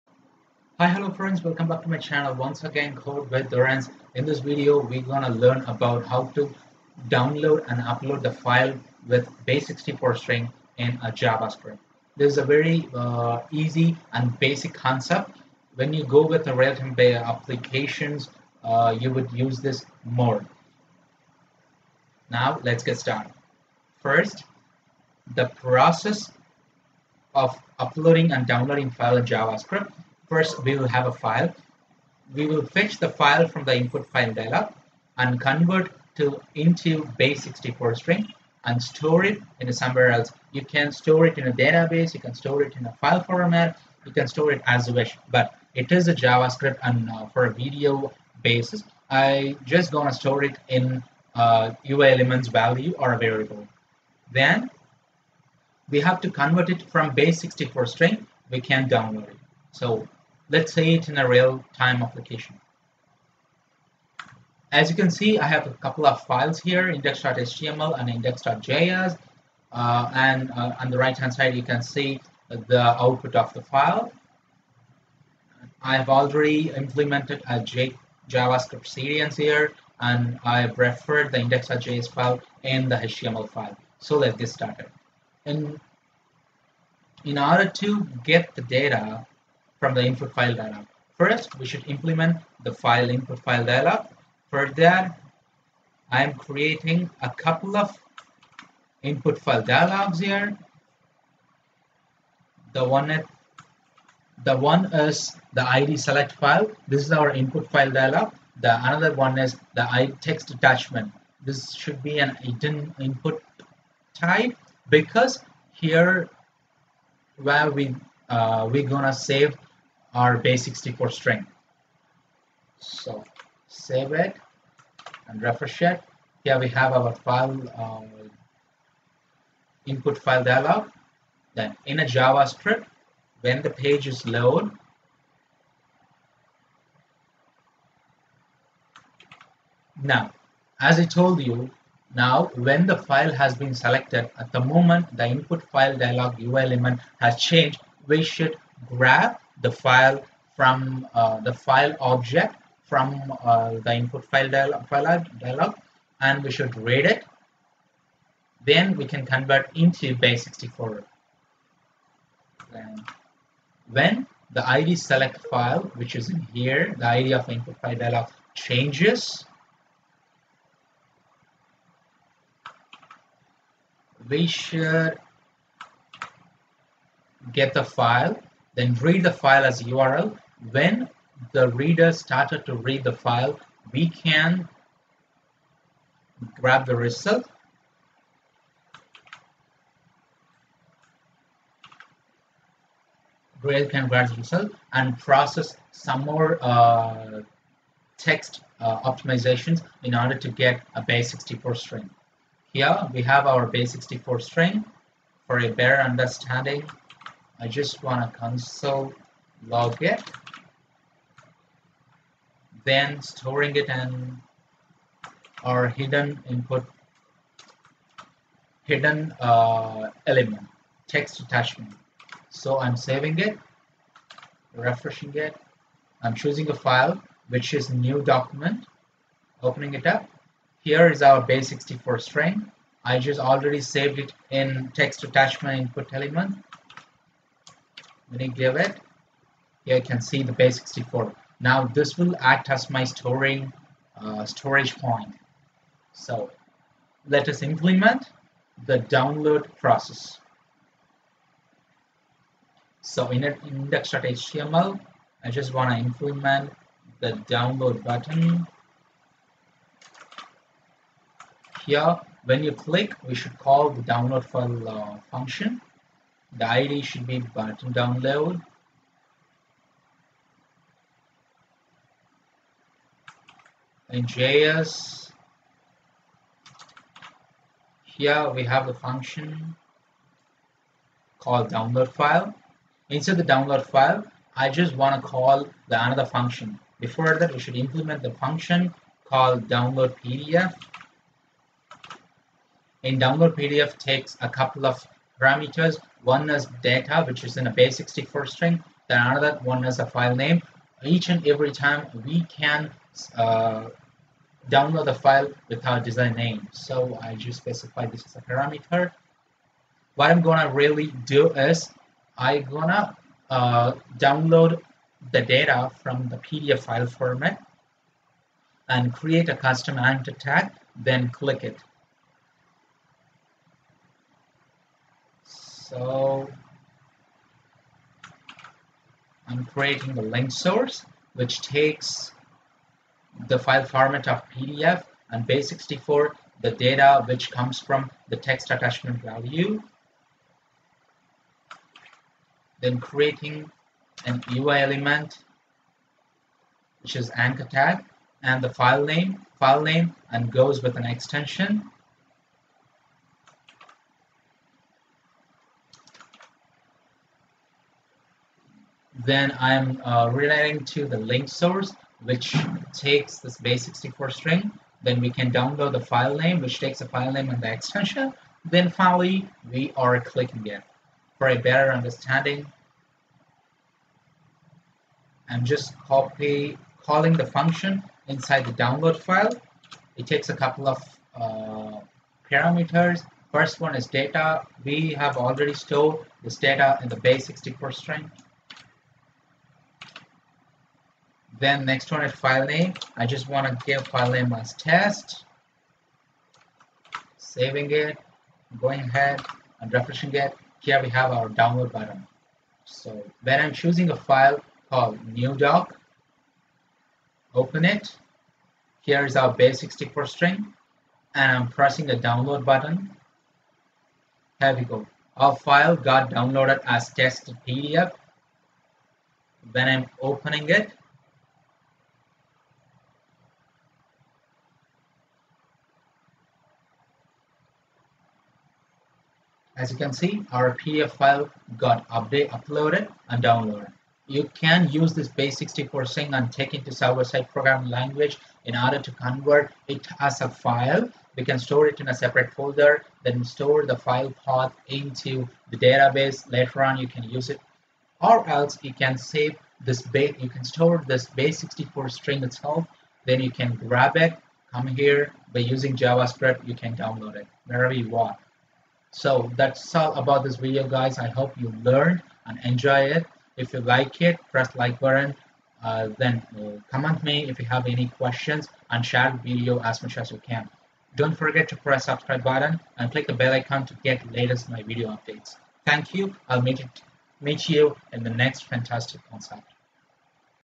Upload and Download file with base64string in Javascript | Dharanz | English
Learn about how to upload the file and covert into base64string, then how to download from base64string in Javascript along with the concepts and code.